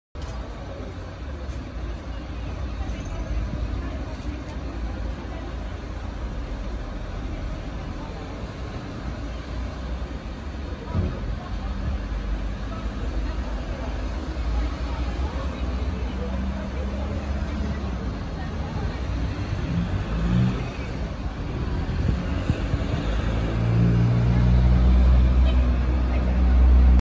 Accident pe trecerea de pietoni
Uite asa te omoara ambulanta pe trecerea de pietoni 😡😡😡